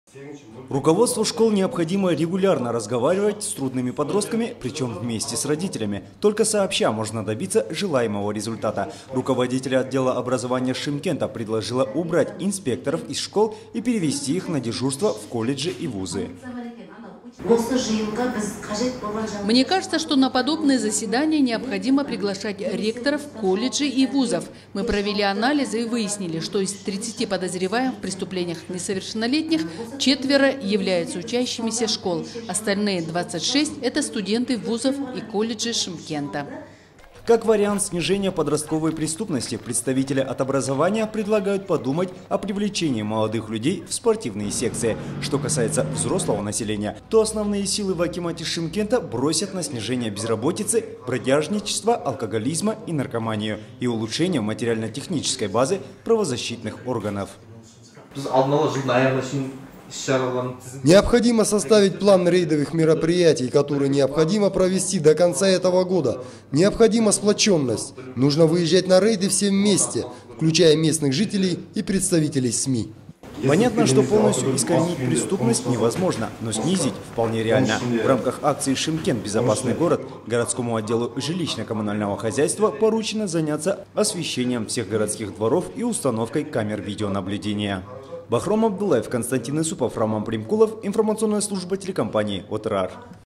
Руководитель отдела образования Шымкента предложила убрать инспекторов из школ
В акимате города Шымкент прошло первое заседание, в рамках которого были обсуждены вопросы проведения, утвержденной акимом города, совместно с главой региона, дорожной карты «Шымкент – безопасный город».